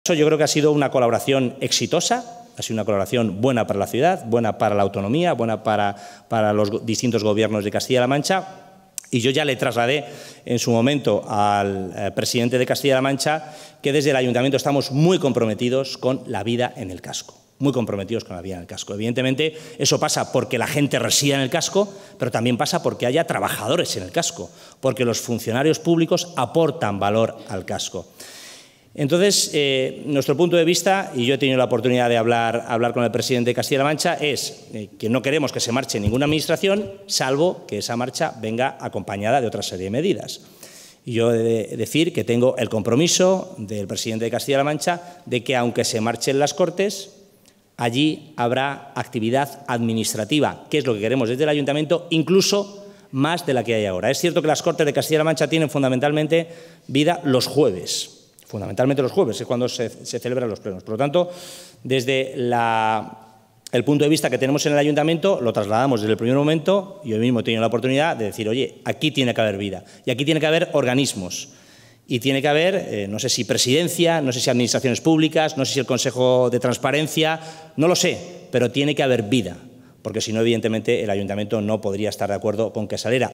Ayuntamiento Toledo no se opondrá al traslado de las Cortes
El alcalde de Toledo, Carlos Velázquez, ha afirmado este miércoles que el Ayuntamiento capitalino no "pondrá problemas" al traslado de la sede de las Cortes de Castilla-La Mancha al edificio del Quixote Crea, siempre y cuando en el Convento de San Gil se mantenga la vida administrativa.